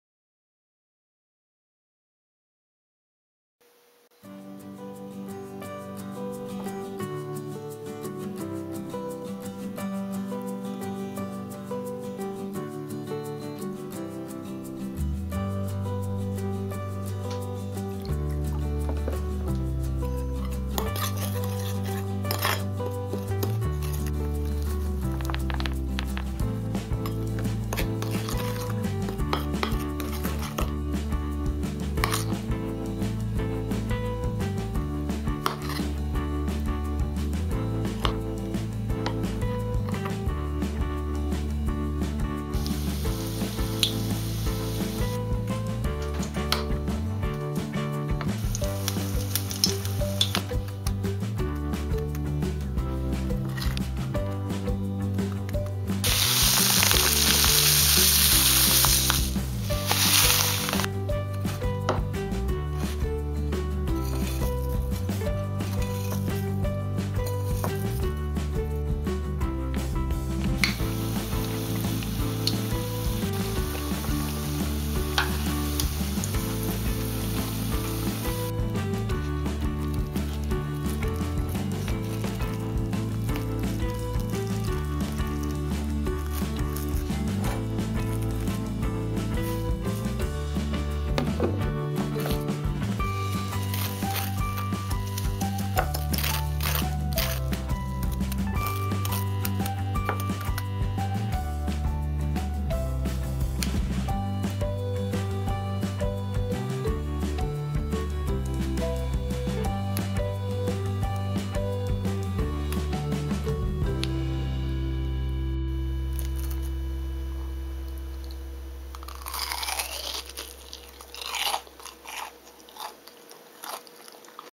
[몽브셰] 바삭바삭 핫도그 만들기 (hot dog)
안녕하세요,몽브셰입니다:)

바삭바삭한 핫도그 만들기~핫케이크 가루로 만들어 봤어요!
마요네즈를 처음 뿌려봤는데 고소한 맛도 나고 괜찮은거 같네요~ㅎㅎ